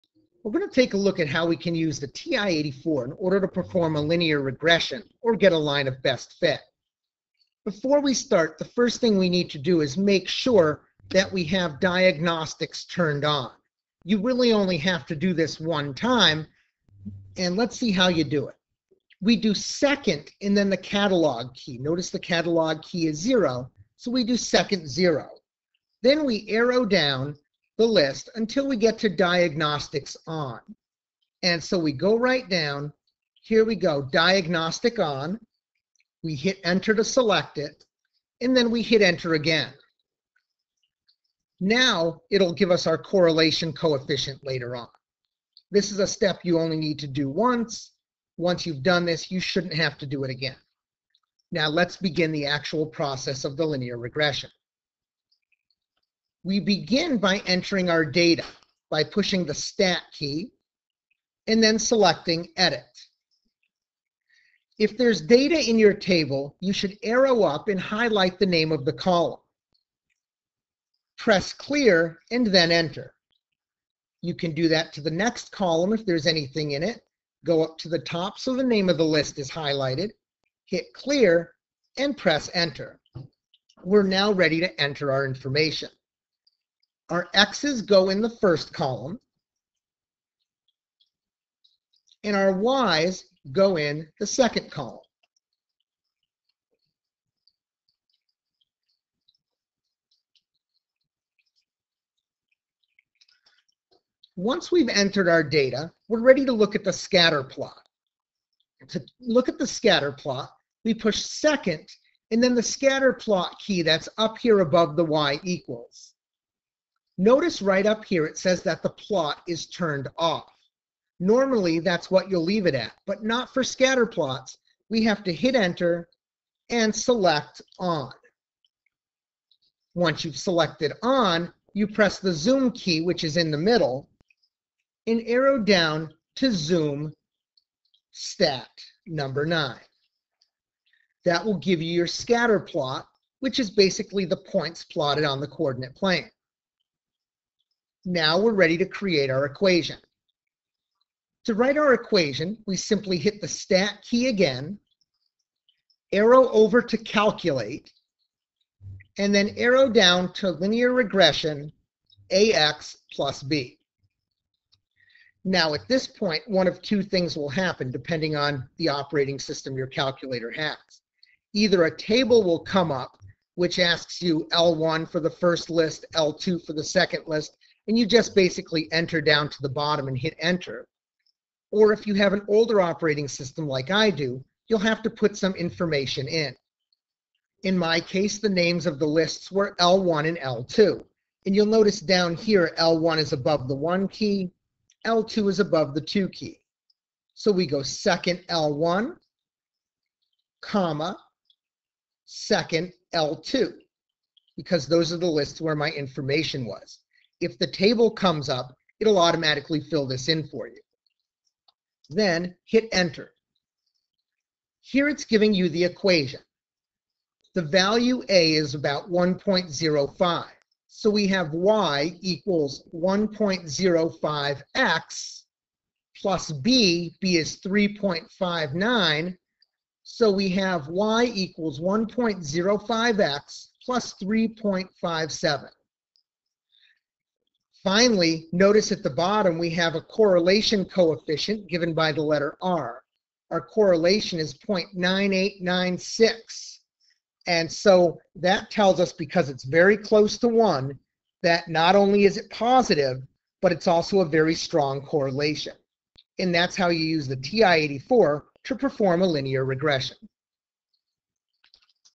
Linear Regression - TI-84
How to perform a linear regression analysis using the TI-83/TI-84 Check out Mr. Dorey's Algebra Handbook - A comprehensive guide and handbook for Algebra students.